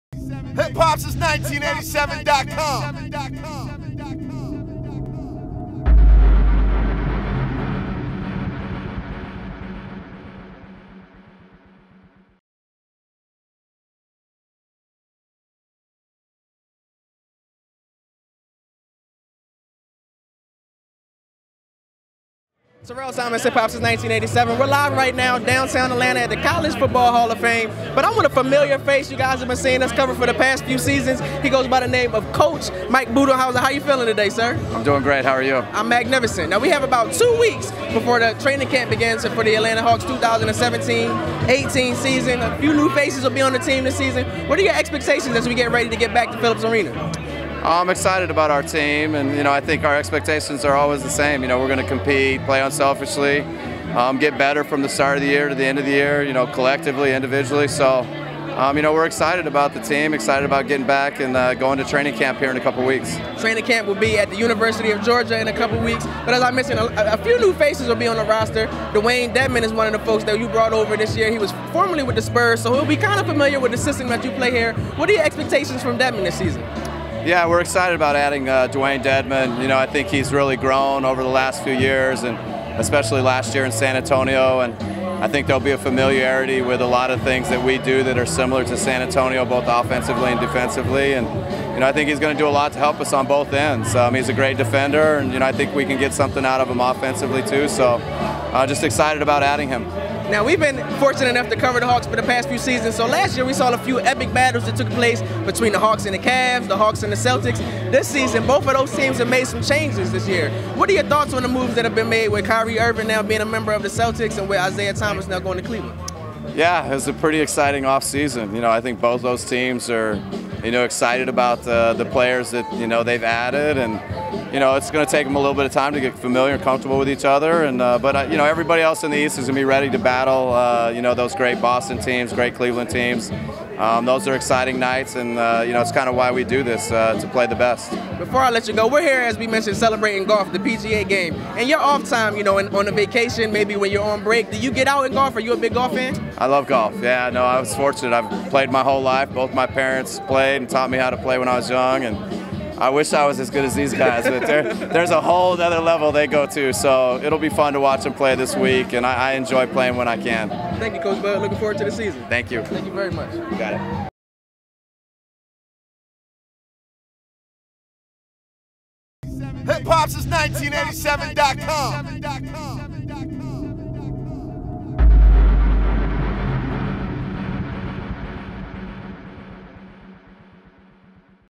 Atlanta Hawks Mike Budenholzer Talks New Season & More (Atlanta Celebrates the TOUR Championship)
On Monday September 18th, the 3rd annual PGA Tour "Atlanta Celebrates the TOUR Championship" event took at the College Football Hall of Fame in downtown Atlanta. The event brought together the Atlanta business community along with some of Atlanta biggest tastemakers to kick-off TOUR Championship week, now in its 16th consecutive year in Atlanta.

The event was hosted by V103 morning show host Ryan Cameron and the evening featured a performance by hometown favorite, Ludacris. The community celebration honored the PGA TOUR's top 30 players and included appearances by Georgia Tech alum Matt Kuchar, Jon Rahm, Patrick Reed, Daniel Berger, Pat Perez and Brian Harman.

To gear fans and Atlanta residents up for the Tour Championship which takes place at the East Lake Country Club from September 21st-24th, many of the Atlanta Hawks and Atlanta Falcons were in the building. While walking the red carpet, HHS1987's own Terrell Thomas had a chance to speak with Atlanta Hawks head coach Mike Budenholzer.

Just two weeks away from the start of Hawks training camp, Coach Bud spoke on his excitement for the season and he delivered some insight on his goals for the teams. With the addition of big man Dewayne Dedmon and a few other key player this offseason, Coach Bud spoke on the new Hawks roster and how they will gel together this season. Before closing, we also spoke with Coach Bud on the new look Eastern Conference. With Derrick Rose, Gordon Hayward, Kyrie and Dwight Howard all in new uniforms in the East, Coach Bud spoke on the 2017-18 Eastern Conference and his love for the PGA and the game of Golf.

The interview was conducted by Terrell Thomas. The interview was shot by Danny Digital.